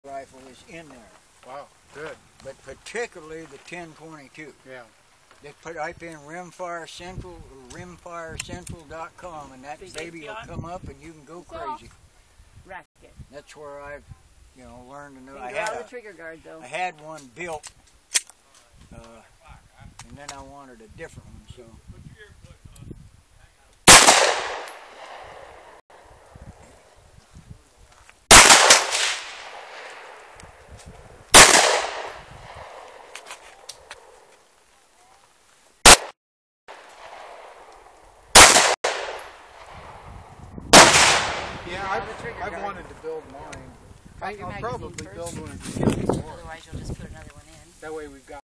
Emma at Urban 3 gun ..Pistol stage
Emma at the urban 3 gun pistol stage. Taurus PT 24/7 Pro ds 9mm / Williams fire sights